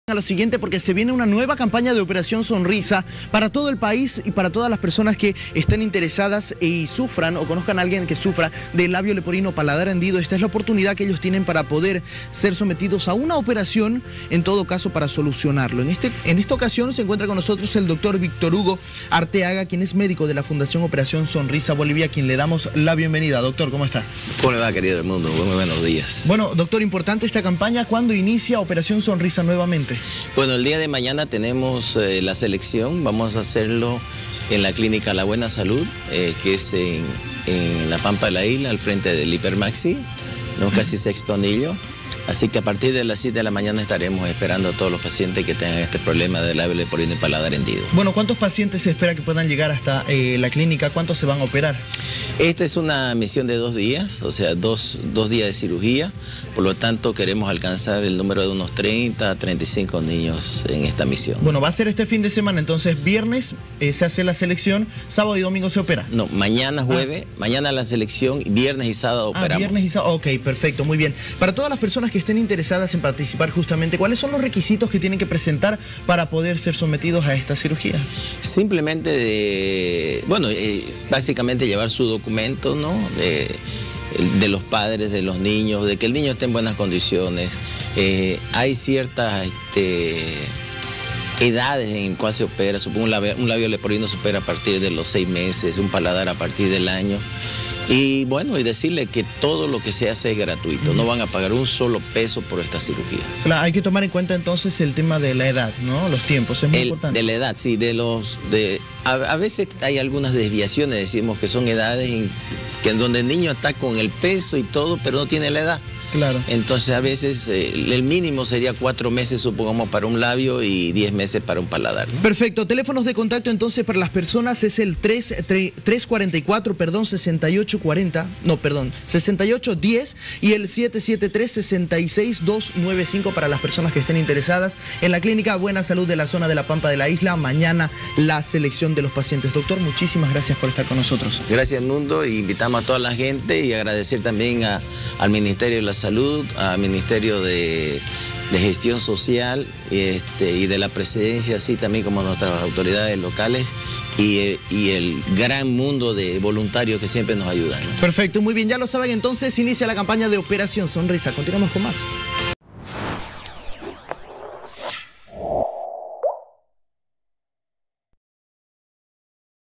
Convocan a pacientes para ''Operación Sonrisa''
En el espacio de entrevista nos visita el miembro de la organización de ''Operación Sonrisa'', quien nos habla en detalle sobre esta iniciativa y la convocatoria para los pacientes que quieran acceder a estas operaciones.